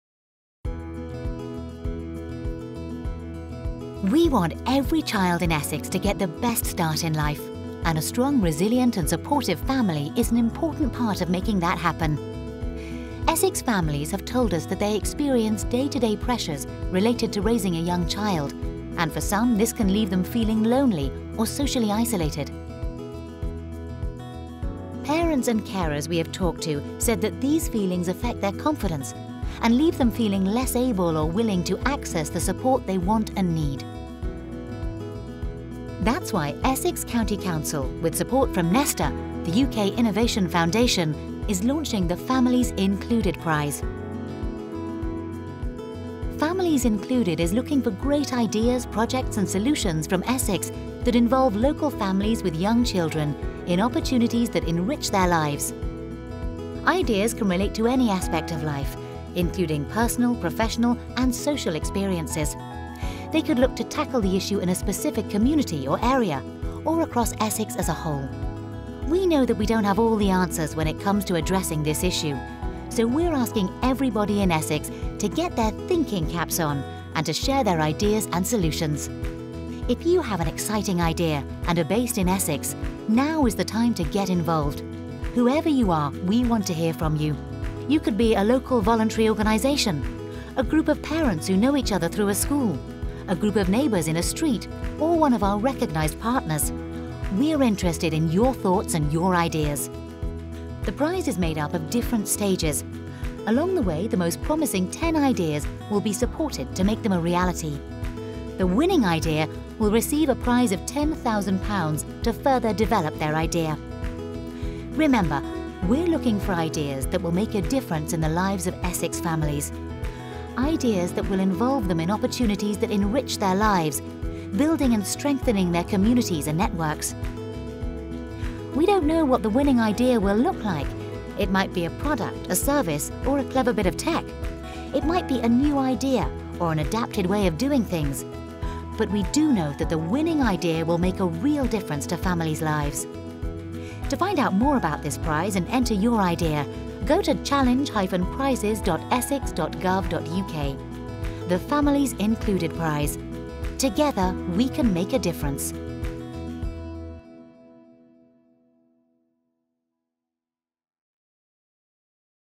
What is the Families Included Prize and how can I enter?
Families Included is looking for great ideas, projects and solutions from Essex that involve local families with young children in opportunities that enrich their lives. Visit challenge-prizes.essex.gov.uk to find out more.